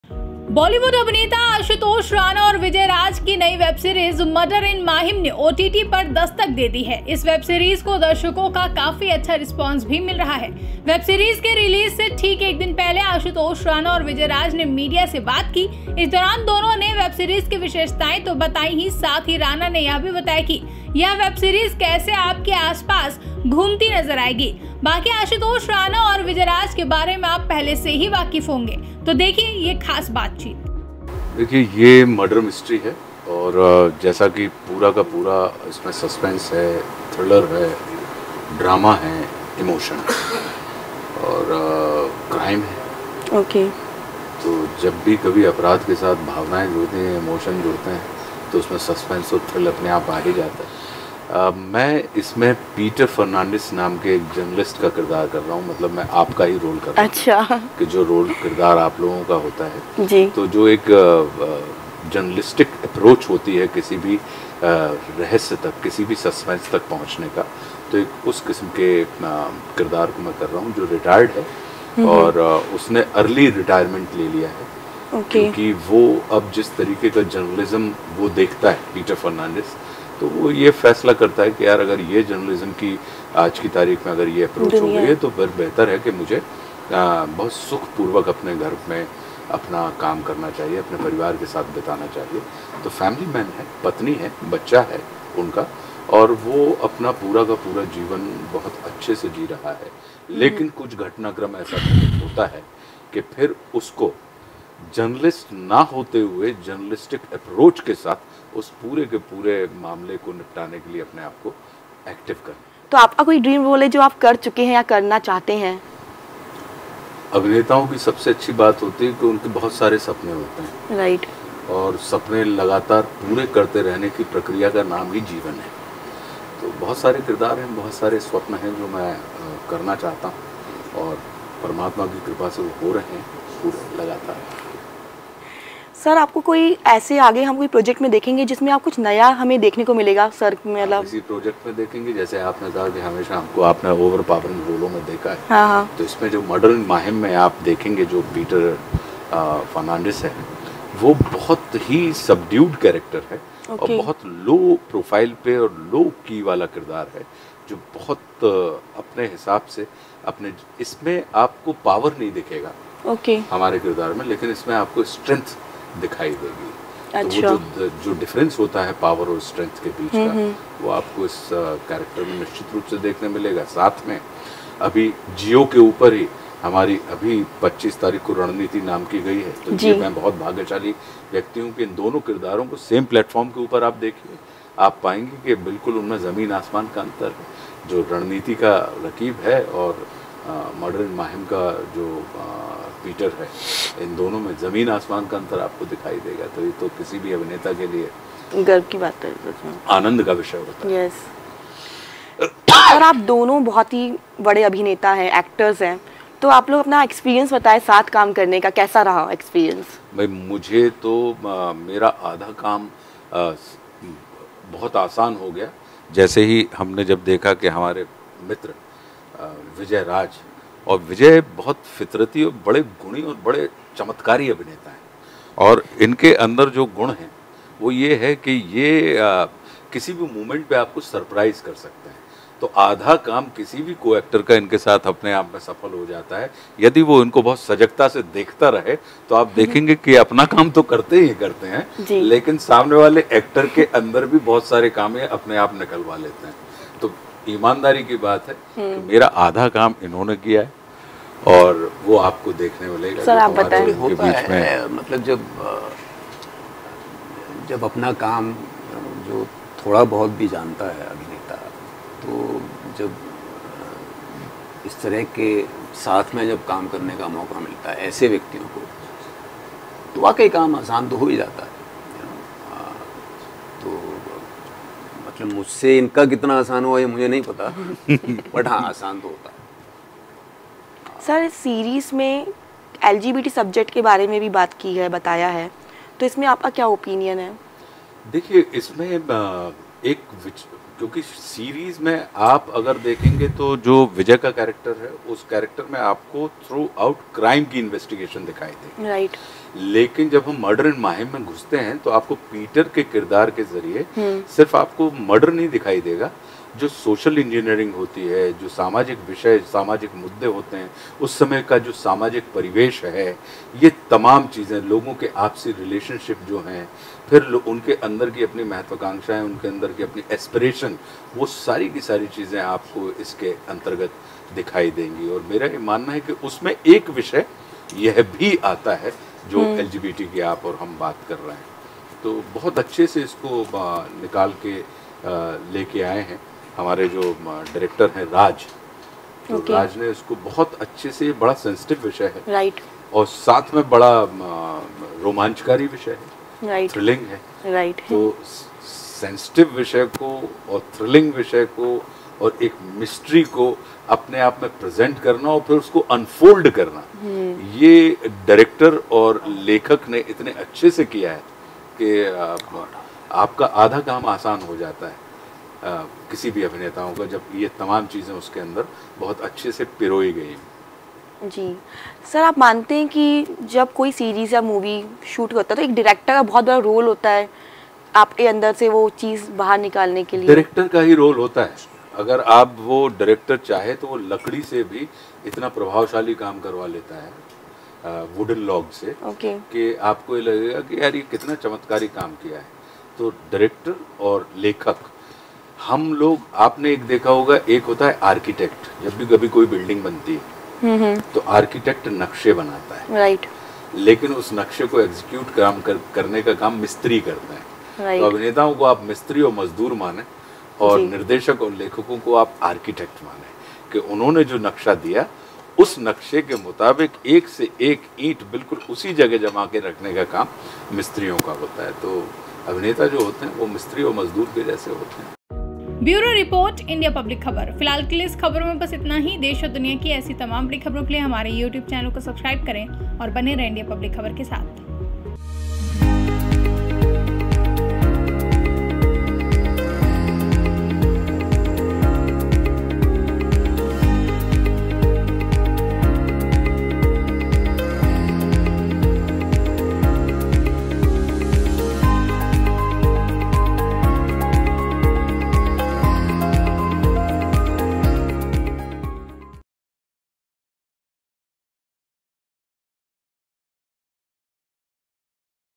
Murder In Mahim में एक ख़ास और अलग Charecter में नज़र आएंगे Ashutosh Rana!
Bollywood actor Ashutosh Rana and Vijay Raj's new web series Murder in Mahim has hit the OTT...This webseries is also getting a very good response from the audience...Just a day before the release of the webseries, Ashutosh Rana and Vijayraj talked to the media... During this, both of them not only told the features of the web series but Rana also told how this web series will be seen revolving around you...

About Channel